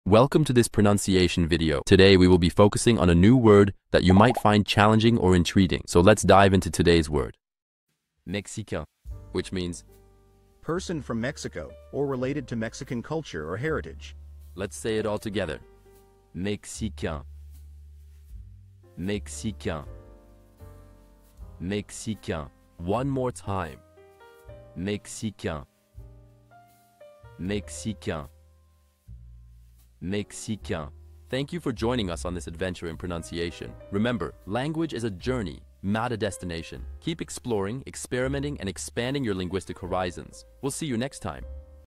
How to pronounce Mexicain
Master the Pronunciation of 'Mexicain Which Mexican Means' - which means : Person from Mexico City, or related to Mexican Culture or Heritage. 📝 with @BaguettePronunciation 🎓🗣️ - [Your Guide to French]

Learn to pronounce 'Mexicain' perfectly in French with our easy and engaging tutorial by @BaguettePronunciation! 📖📽️ In this comprehensive video, we provide you with native speaker examples 🗣️👂 and phonetic breakdowns 🎵 to ensure you get the pronunciation just right.

How to say Mexican in French